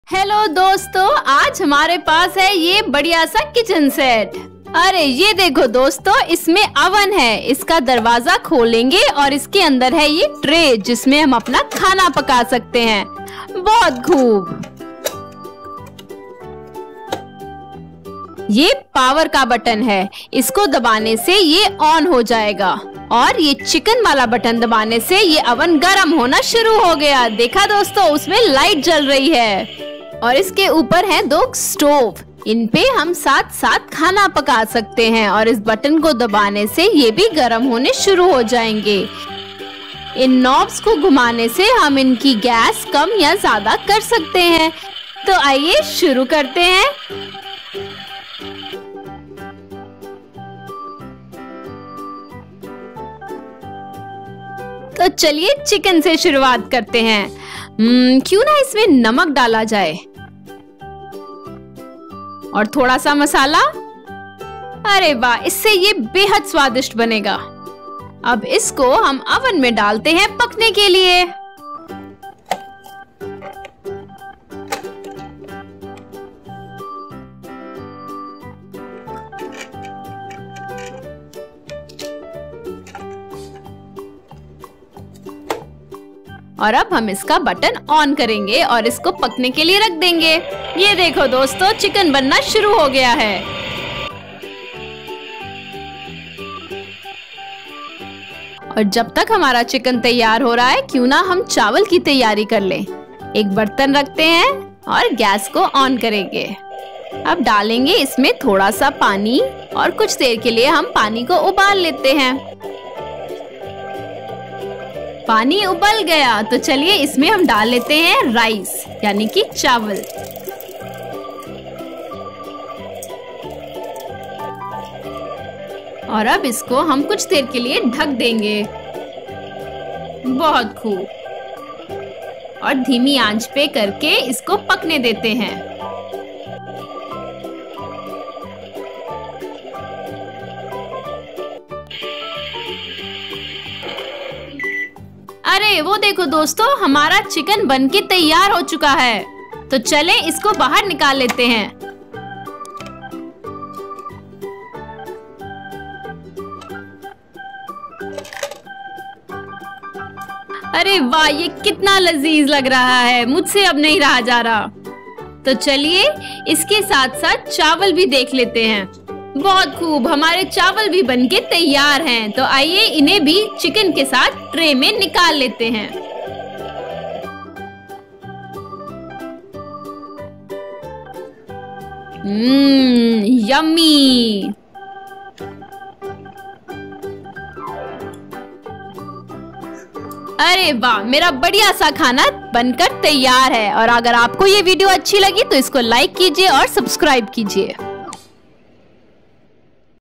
Kitchen Toy Play | रसोई खिलौना सेट | Microwave Oven | TOYHOUSE STORIES - हिंदी में
माइक्रोवेव खिलौना ओवन सेट के साथ कुकिंग का मजेदार अनुभव | इस वीडियो में माइक्रोवेव खिलौना ओवन सेट कैसे काम करता है, जिससे वे खेल-खेल में खाना बनाने का अनुभव ले सकते हैं। देखिए कैसे हम इस माइक्रोवेव खिलौने का उपयोग करके प्रीटेंड फूड पकाते हैं और मजेदार डिश तैयार करते हैं। यह खिलौना बच्चों को किचन एक्टिविटी के लिए प्रेरित करता है, जहां वे अपनी खुद की कहानियाँ और डिशेज बना सकते हैं। यह वीडियो प्रीस्कूलर्स और टॉडलर्स के लिए आदर्श है, जो खेल-खेल में नई चीजें सीखते हैं।

© TOYHOUSE STORIES - हिंदी में